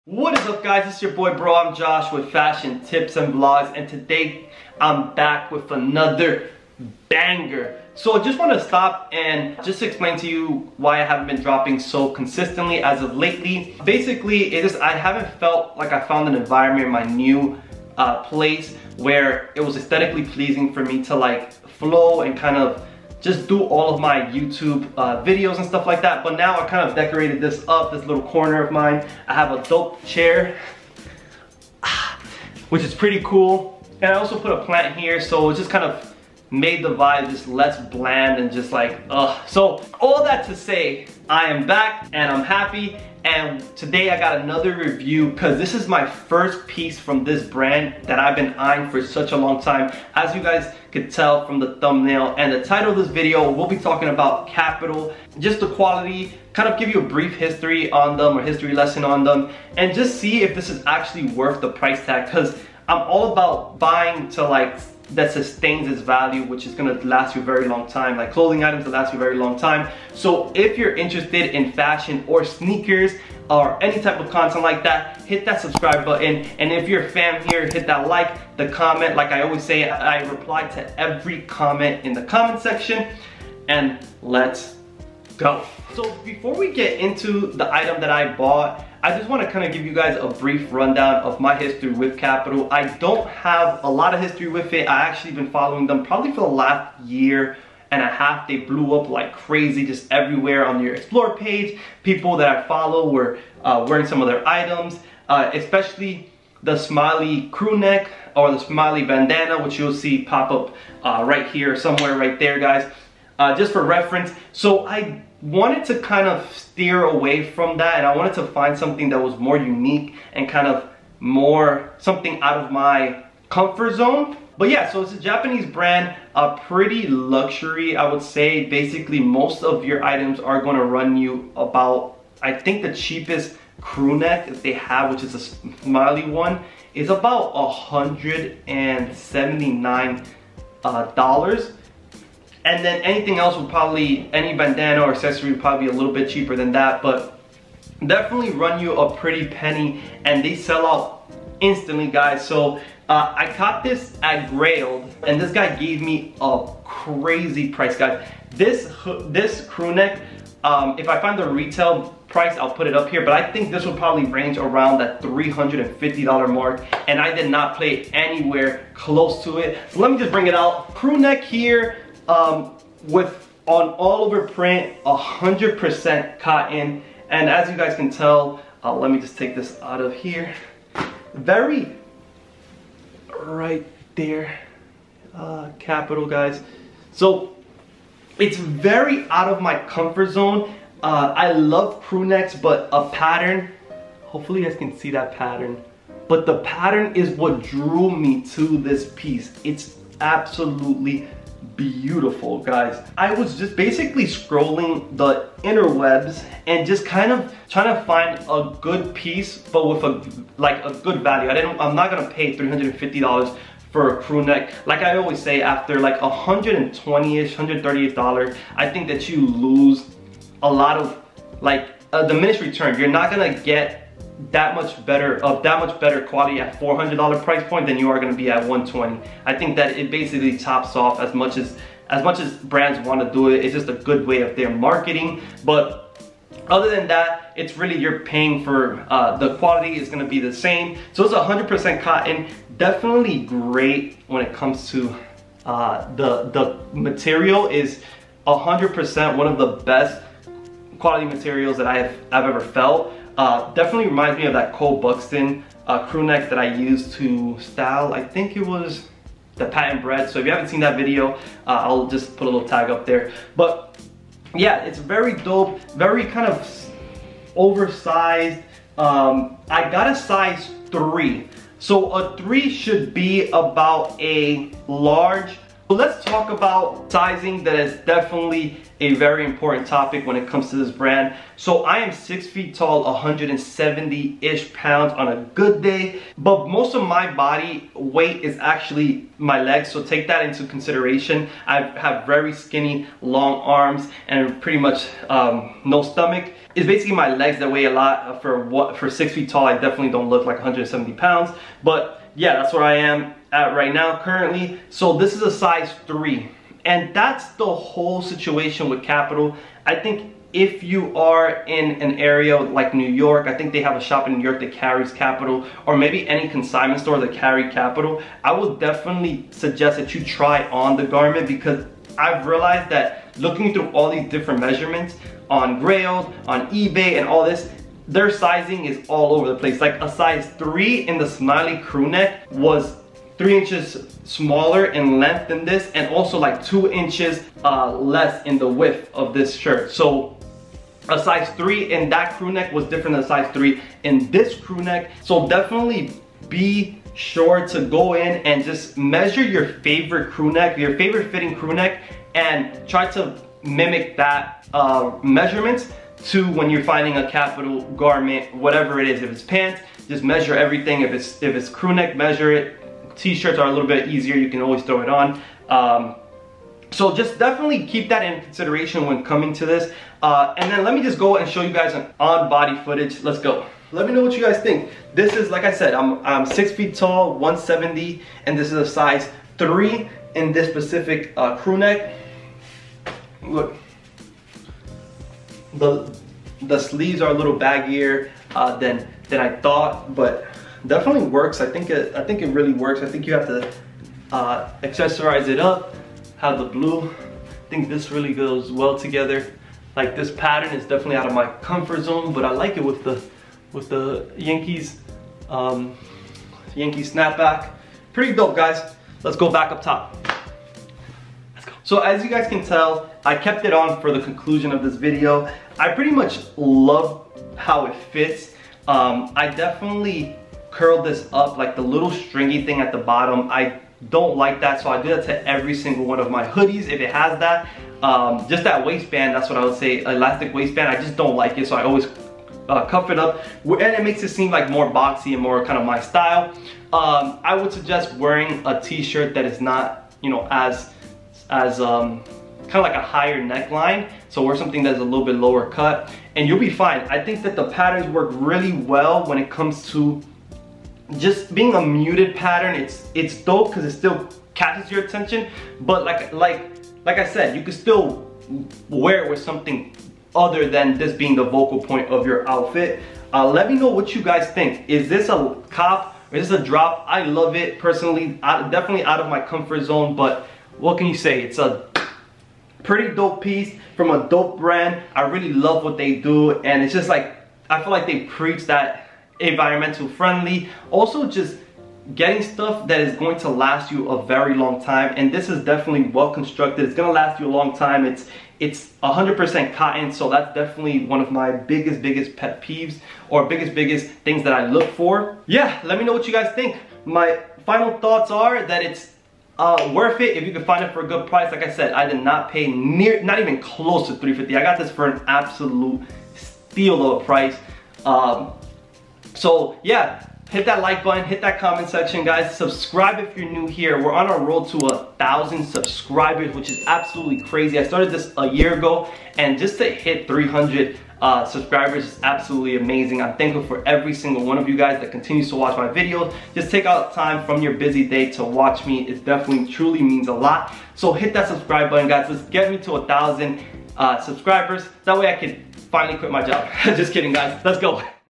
EVERYTHING YOU NEED TO KNOW ABOUT KAPITAL | REVIEW | SIZING & MORE
In this video, I review a Kapital Heather Fleece "Octopus Arabesque" crewneck that released last year. I bread down the details of this crewneck/ sweatshirt from the materials to the fit. Lastly, I show you an on body look.

Thank you for watchig.